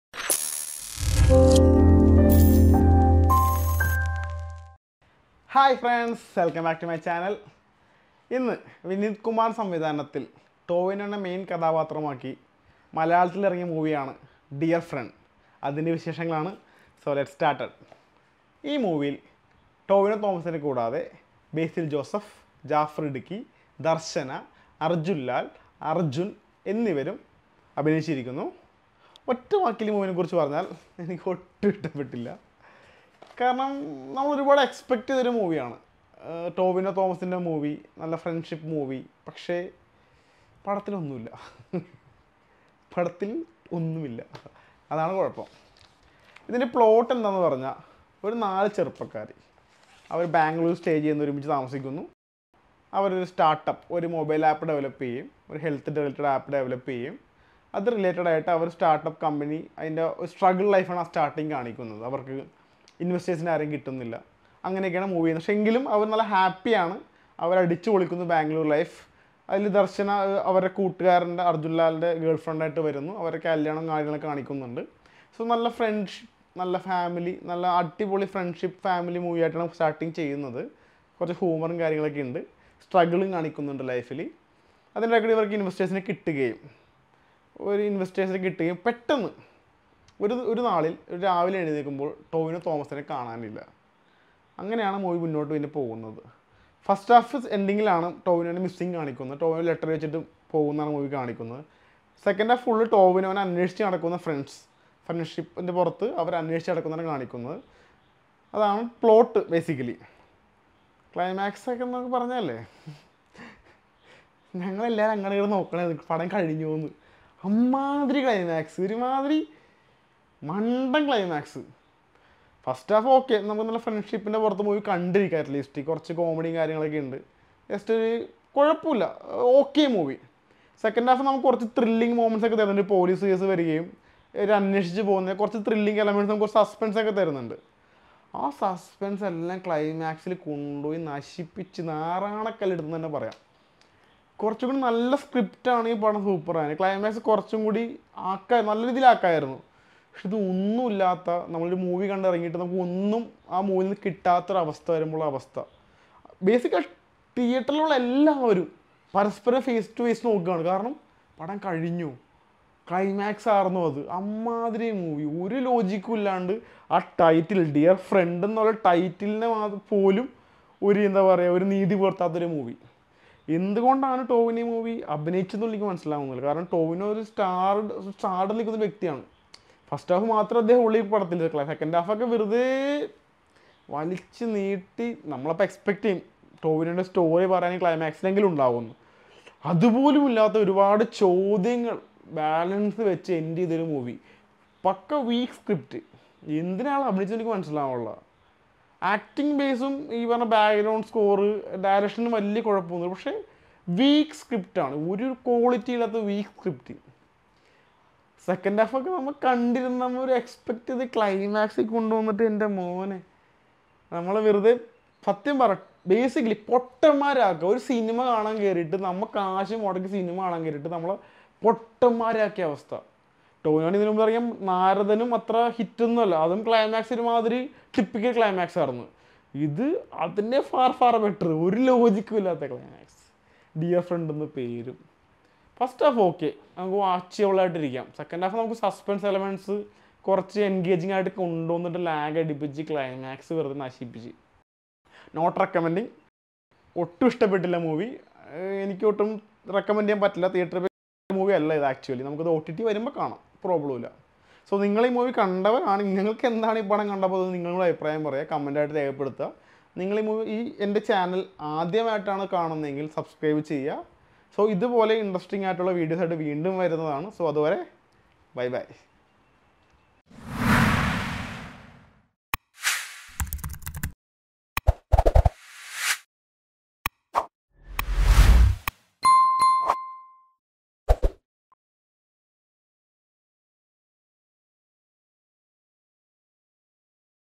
Dear Friend Malayalam Movie Review - സിനിമ വിശേഷങ്ങൾ
Dear Friend is a drama film directed by Vineeth Kumar, featuring Tovino Thomas and Darsana Rajendran in prominent roles.